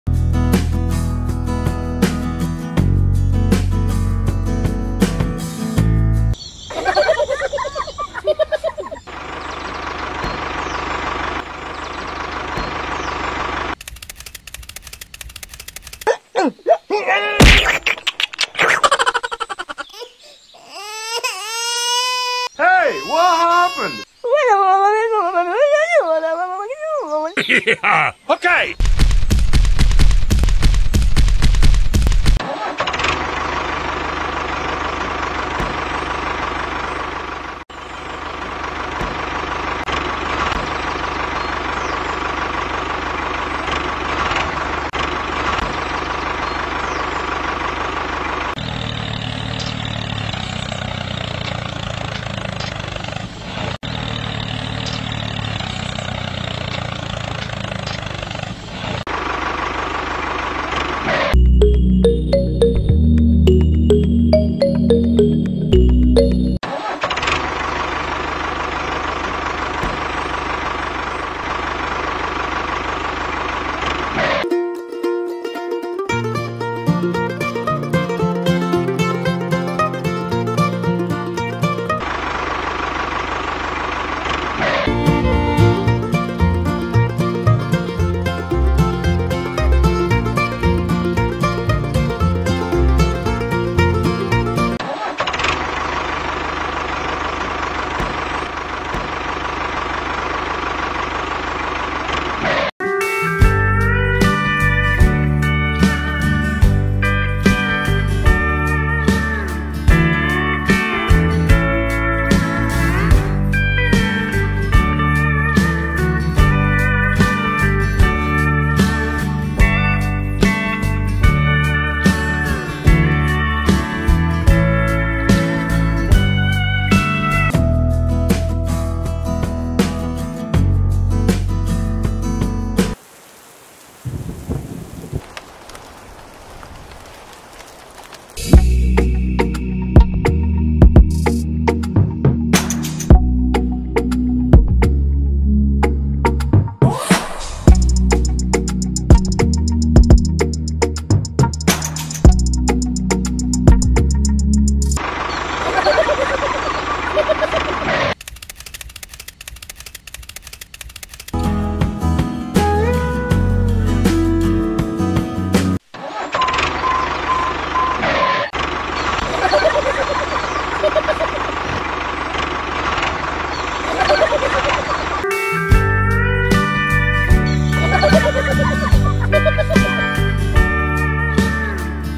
Diy mini tractor making modern agriculture plough machine for grape farming
Hello all every one. Please Watch our video.
◕‿◕ Have fun watching the video everyone!